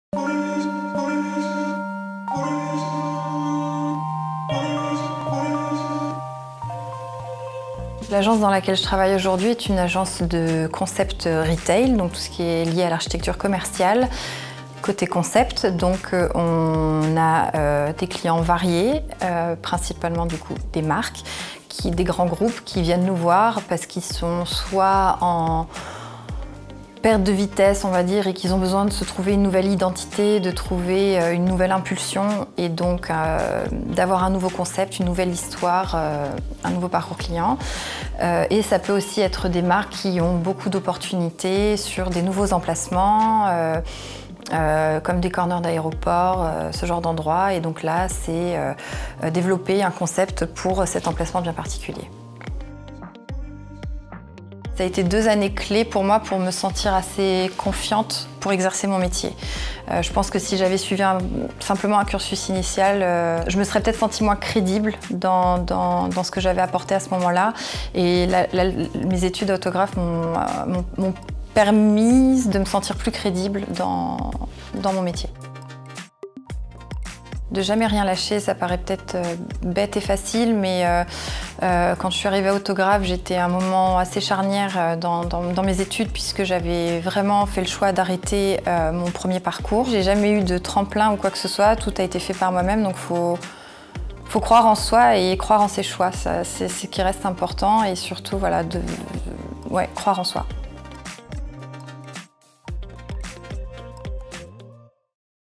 Cindy MANSCOURT - ANCIENNE ÉTUDIANTE EN MASTÈRE D’ARCHITECTE DESIGNER D’INTÉRIEUR
Cindy MANSCOURT est aujourd’hui Architecte d’intérieur chez MALHERBE PARIS. Ancienne étudiante en Architecture d’Intérieur, Cindy a suivi son Mastère en alternance dans notre école. Cindy a accepté de répondre à nos questions et témoigne de sa formation à Autograf, de son métier...